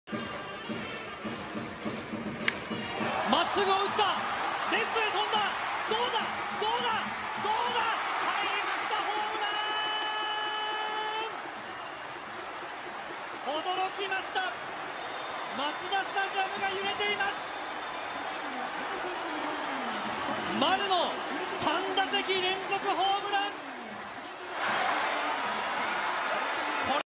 丸佳浩 3打席連続第11号ソロホームラン
2017／06／16 広島vsソフトバンク